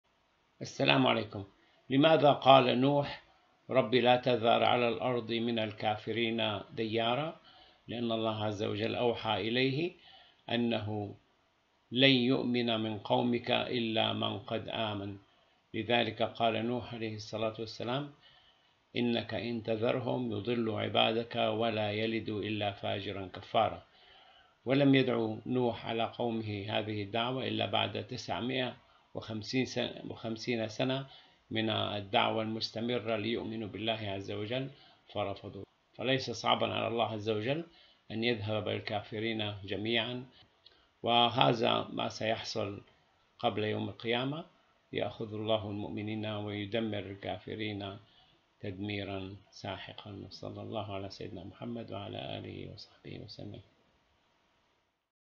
Why prophet Noah AS asked
Why prophet Noah AS asked Allah to get rid of all unbelievers at his time